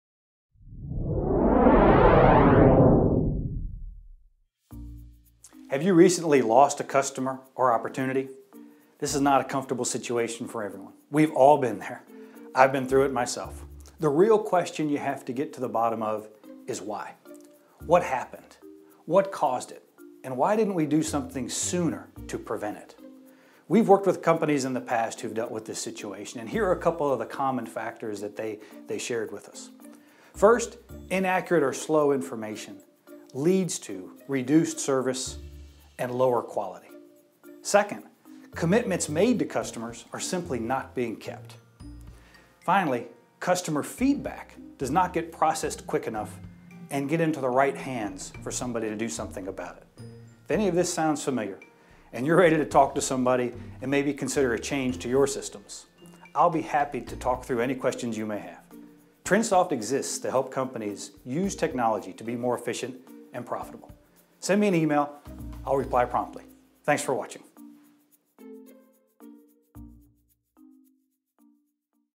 ERP: Loss of Customer
Have you ever lost a customer or opportunity? If it's due to inadequate software, John Stucky with TrinSoft can help.

John Stucky is the Managing Partner at TrinSoft, LLC.